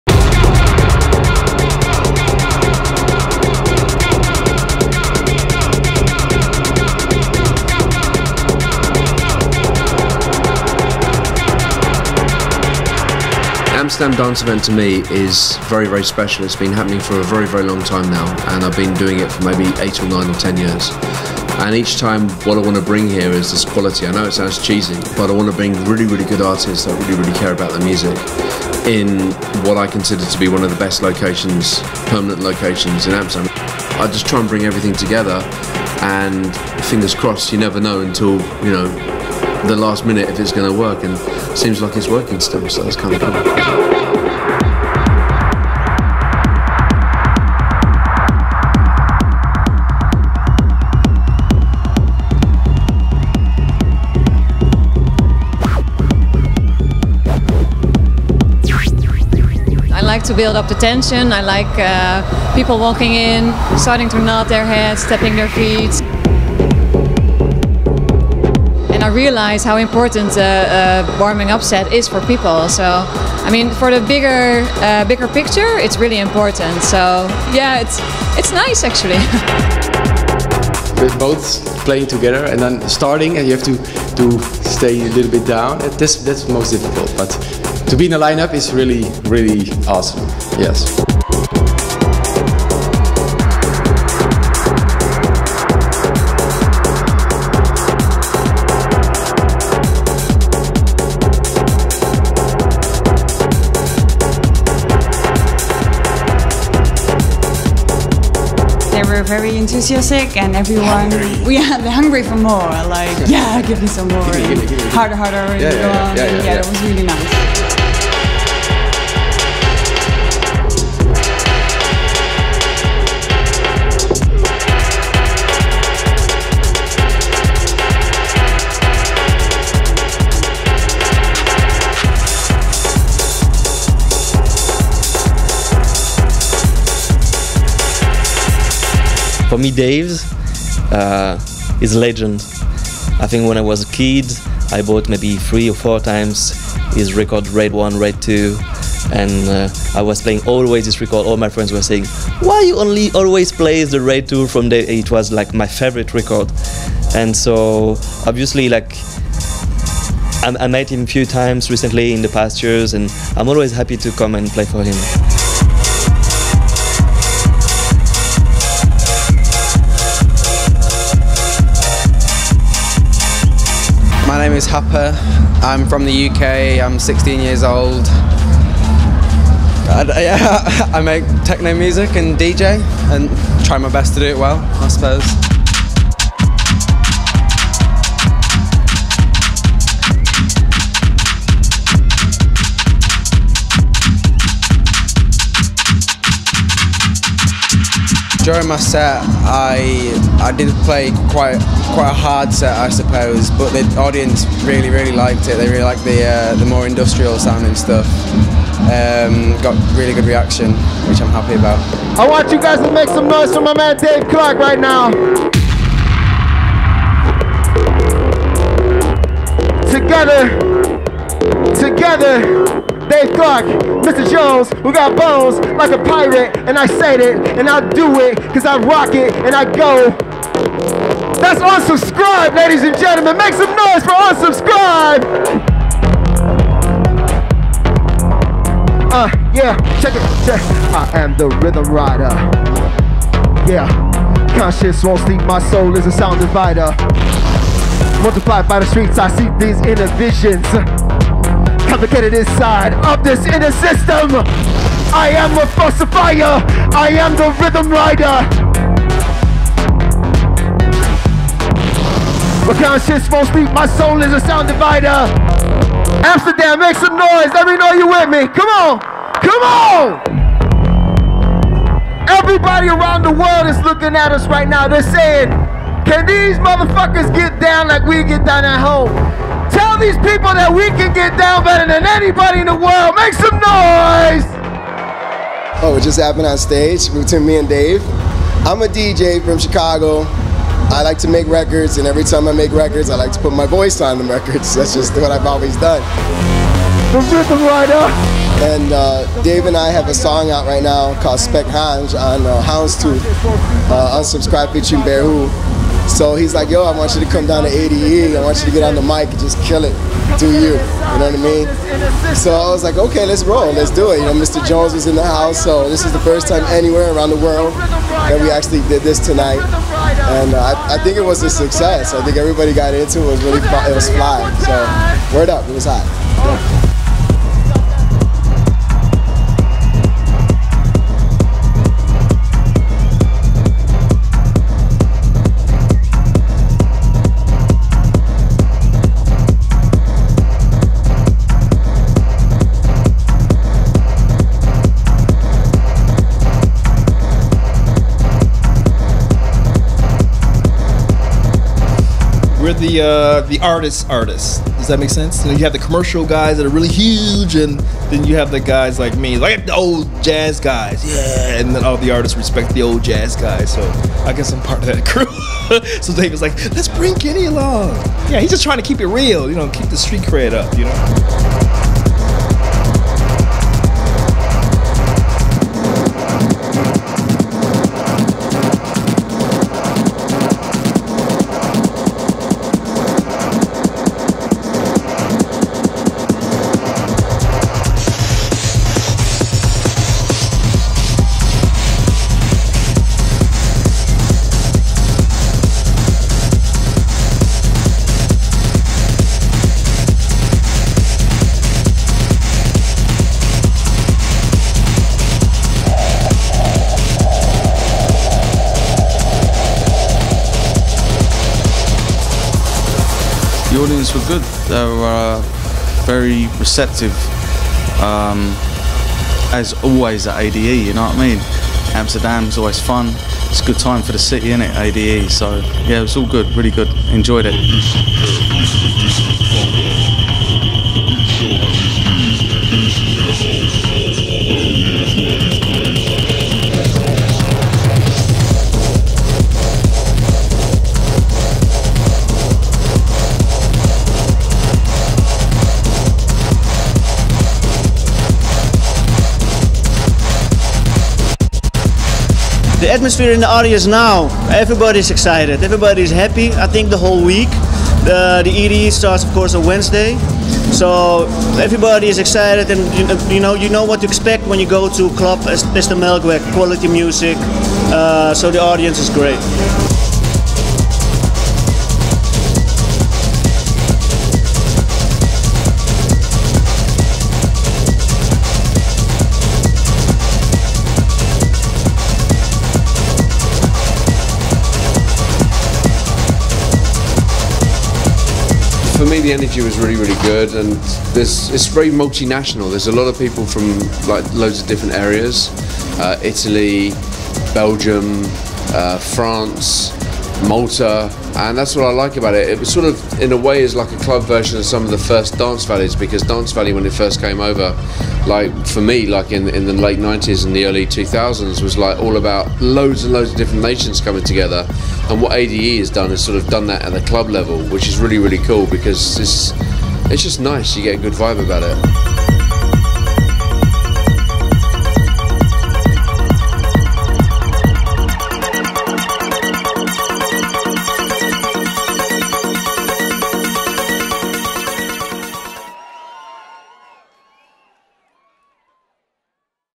Dave Clarke Presents @ ADE 2013 in the Melkweg, Amsterdam.
Dave Clarke Presents @ Melkweg - ADE 2013 showcase [official aftermovie].
DC and the gang once again at a Sold Out edition at the Melkweg Amsterdam, features Bear Who?, Kenny Larkin, Mr. Jones, Agoria, and many more, including yourself if you were there.